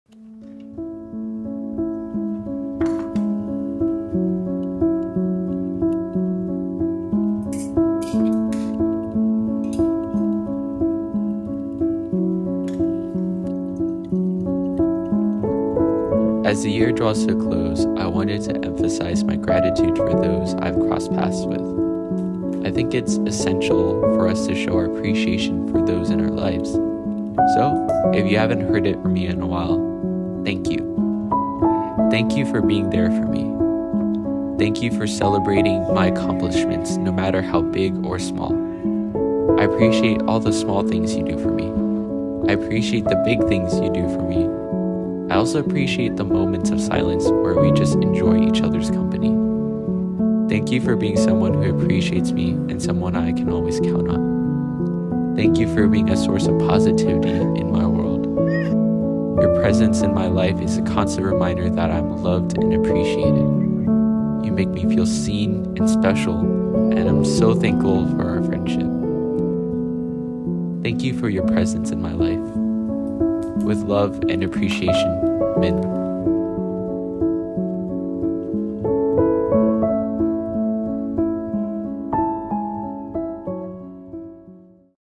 i appreciate you.
As the year comes to a close, I wanted to reflect and have some gratitude for all the people I’ve crossed paths with.

I hope that those who have taken the time to watch my video feel seen, loved and appreciated. Thank you for supporting my journey

With love and appreciation,
Minh